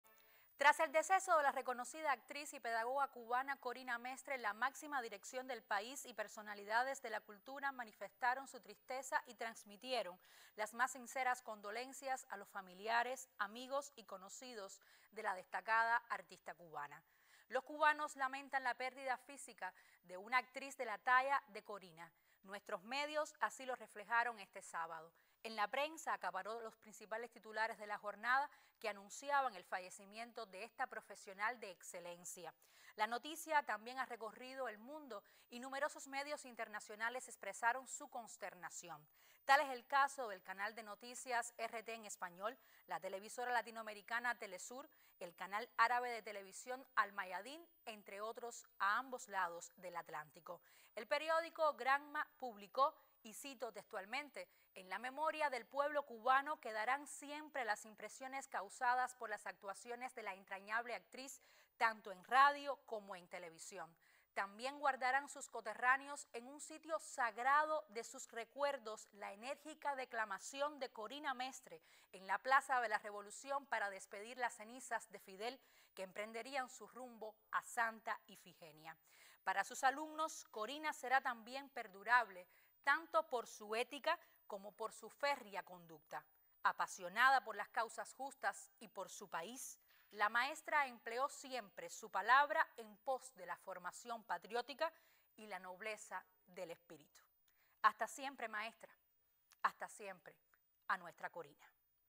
Autoridades de Cuba lamentan fallecimiento de la actriz y pedagoga, Corina Mestre
Tras el deceso de la reconocida actriz y pedagoga de Cuba, Corina Mestre, la máxima dirección del país y personalidades de la Cultura manifestaron su tristeza y trasmitieron las más sinceras condolencias a los familiares, amigos y conocidos de la destacada artista.